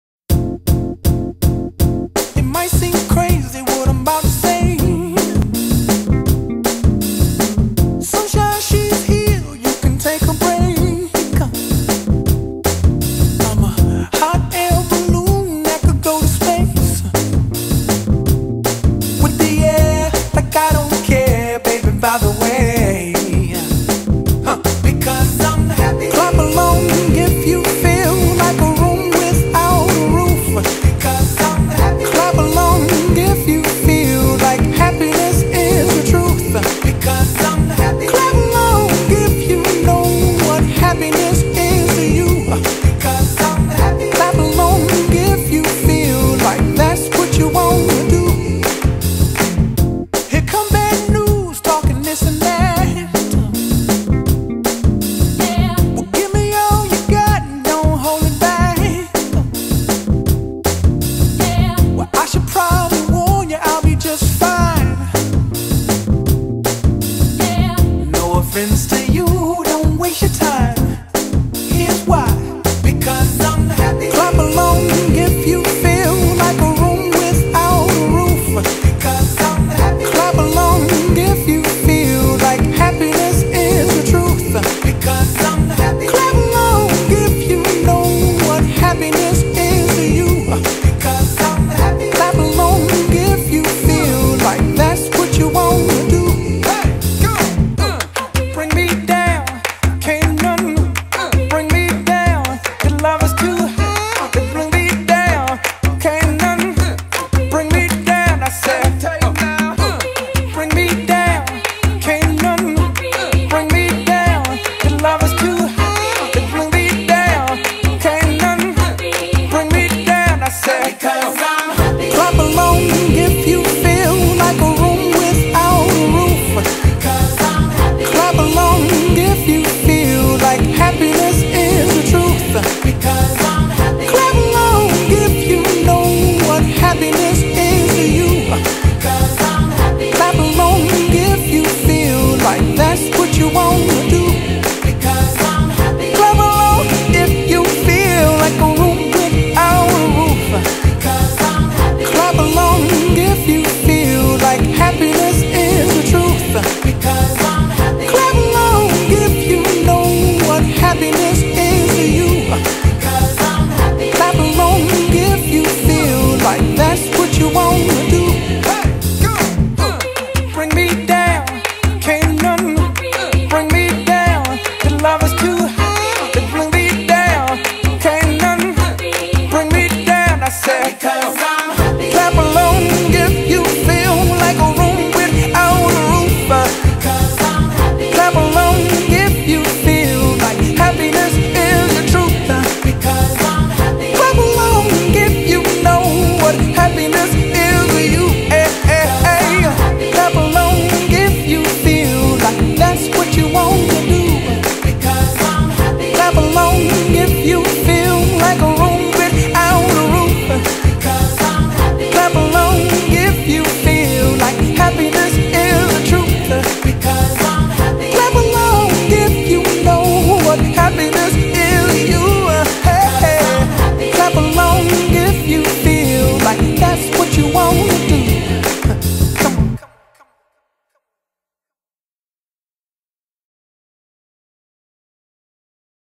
Dim n Wit the Fairy Brothers, Space City Con 2014
Space City Con 2014 through the eyes of Dim N Wit... Hope you are smiling at the end of this..